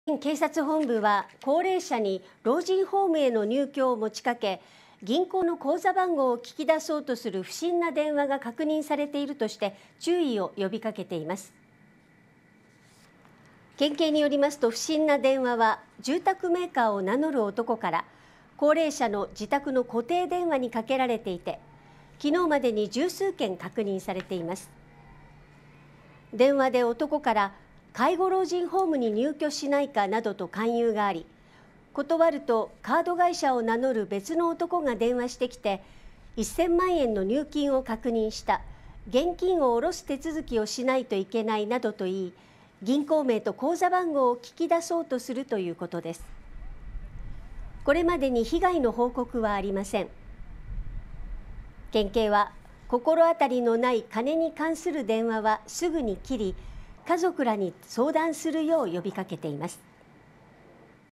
老人ホームへの入居をすすめ口座番号を聞く不審電話に注意 鹿児島（2022.8. 6）
鹿児島県警察本部は、高齢者に老人ホームへの入居を持ちかけ、銀行の口座番号を聞き出そうとする不審な電話が確認されているとして注意を呼びかけています。

鹿児島県警によりますと、不審な電話は住宅メーカーを名乗る男から高齢者の自宅の固定電話にかけられていて５日までに十数件確認されています。

電話で男から「介護老人ホームに入居しないか」などと勧誘があり、断るとカード会社を名乗る別の男が電話してきて「１０００万円の入金を確認した」「現金を下ろす手続きをしないといけない」などと言い、銀行名と口座番号を聞き出そうとするということです。

これまでに被害の報告はありません。

県警は、心当たりのない金に関する電話はすぐに切り、家族らに相談するよう呼びかけています。